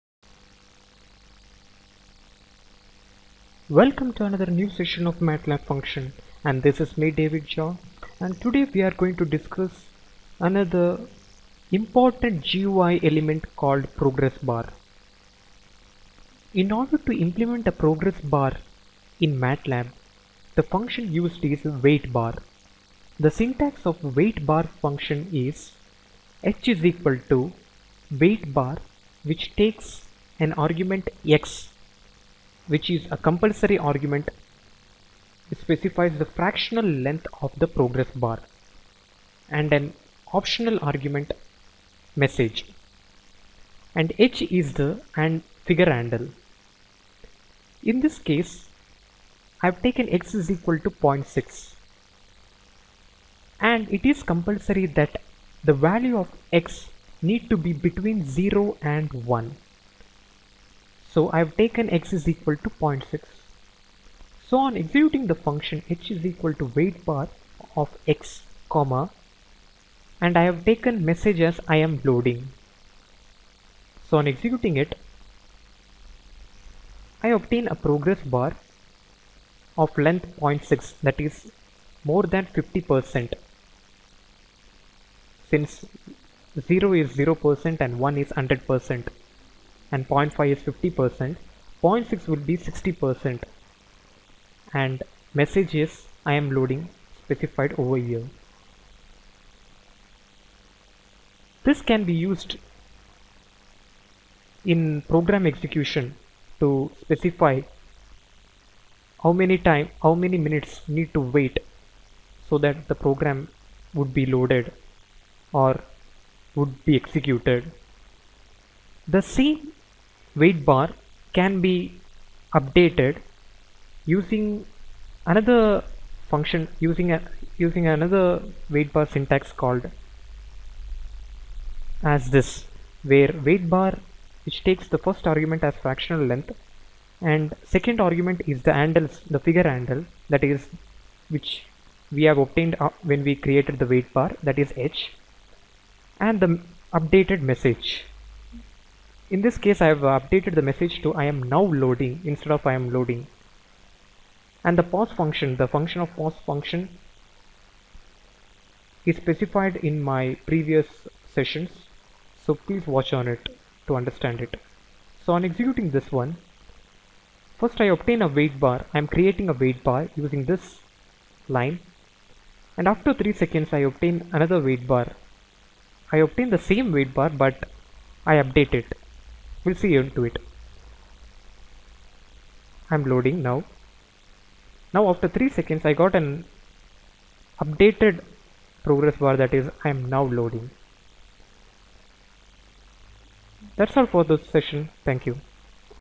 Matlab GUI Function - Progress Bar-waitbar( )
This is a matlab GUI tutorial on waitbar function. waitbar function is used to obtain the create and open progress bar in matlab.